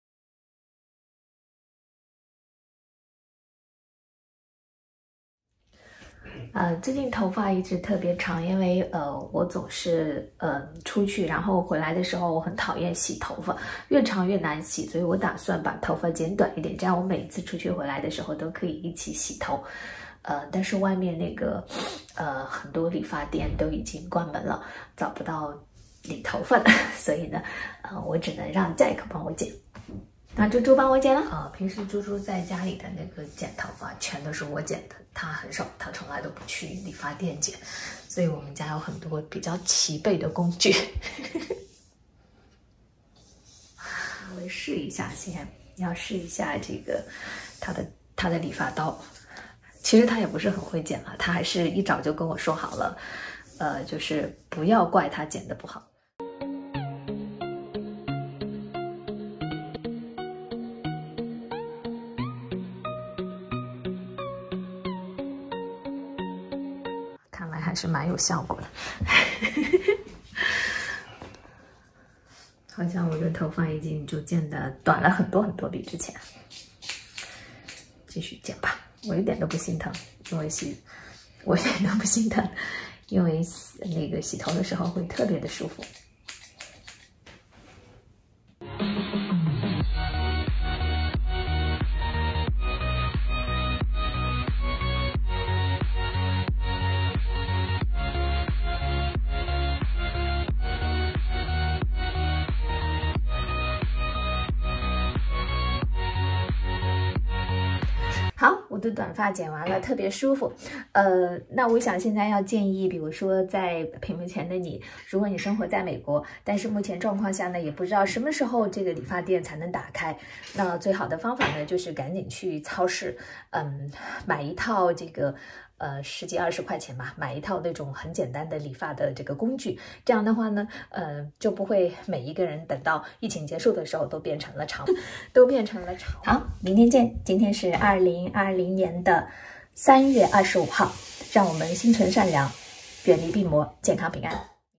3平安vlog /理发店不开门，朱朱帮我剪头发3/25/2020
朱朱帮我剪短发， 工具齐全效果不错哦！
祝福所有人心存善良，远离病魔， 健康平安！